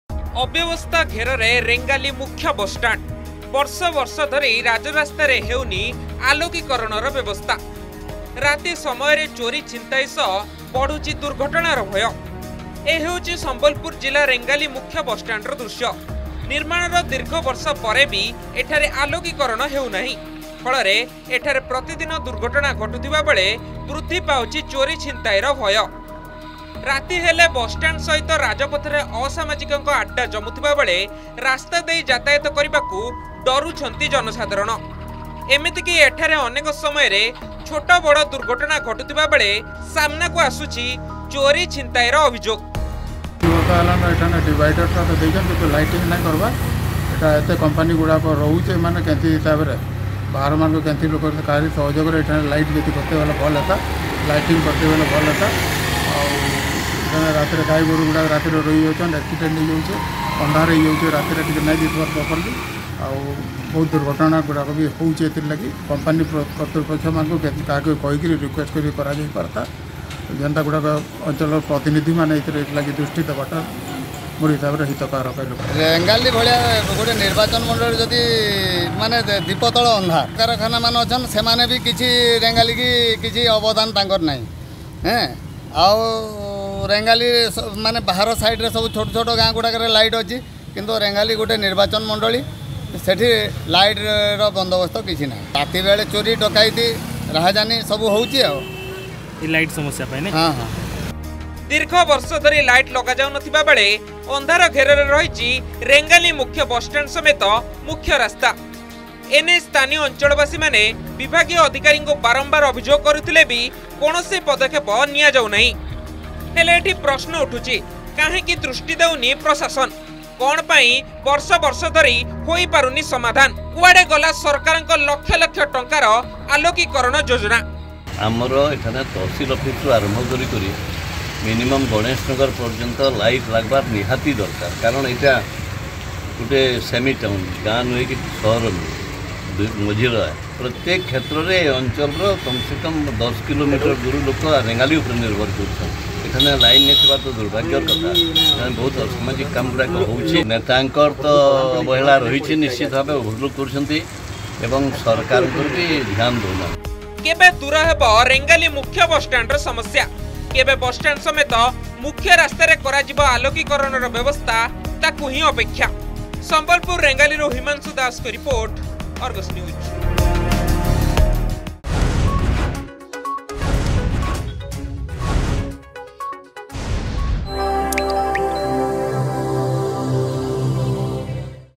Sambalpur: Rengali Main Bus stand in disarray
ସମ୍ବଲପୁର ଜିଲ୍ଲା ରେଙ୍ଗାଲି ମୁଖ୍ୟ ବସଷ୍ଟାଣ୍ଡ ନିର୍ମାଣର ଦୀର୍ଘ ବର୍ଷ ପରେ ବି ଆଲୋକୀକରଣ ହେଉନାହିଁ। ଫଳରେ ଏଠାରେ ପ୍ରତିଦିନ ଦୁର୍ଘଟଣା ଘଟୁଥିବା ବେଳେ ବୃଦ୍ଧି ପାଉଛି ଚୋରି ଛିନତାଇର ଭୟ। ରାତି ହେଲେ ବସ ଷ୍ଟାଣ୍ଡ ସହିତ ରାଜପଥରେ ଅସାମାଜିକଙ୍କ ଆଡ୍ଡା ଜମ୍ମୁଥିବା ବେଳେ ରାସ୍ତା ଦେଇ ଯାତାୟତ କରିବାକୁ ଡରୁଛନ୍ତି ଜନସାଧାରଣ।